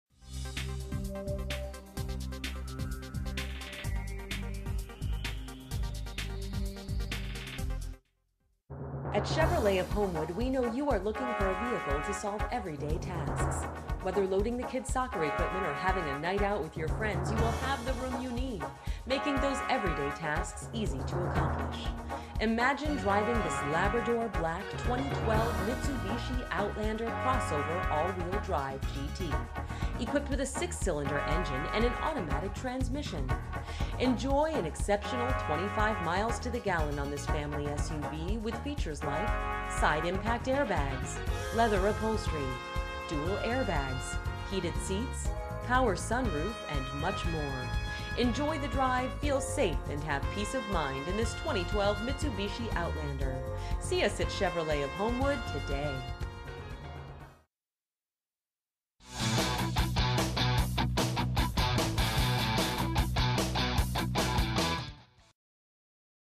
2012 Mitsubishi Outlander Chevrolet of Homewood Homewood, IL 60430
Mitsubishi Outlander At Chevrolet of Homewood, we know you are looking for a vehicle to solve everyday task. Whether loading the kids soccer equipment or having a night out with your friends. You will have the room you need making those everyday task an ease to accomplish. You will love this Labrador Black Pearl Mitsubishi Outlander Crossover AWD, equipped with a 6 Cylinder engine and an automatic transmission. Enjoy an exceptional 25 miles to the gallon on this family SUV with features like Side Impact Air Bags, Leather Upholstery, Dual Air Bags, Heated Seats, Power sunroof, Fog Lights, Alloy Wheels, Single CD Player, Power Seat, and much more. Enjoy the drive, feel safe, and have peace of mind in this Mitsubishi Outlander. See us at Chevrolet of Homewood today!
MILES: 19,733
VIN:JA4JT5AX5CU003665

18033 Halsted ST.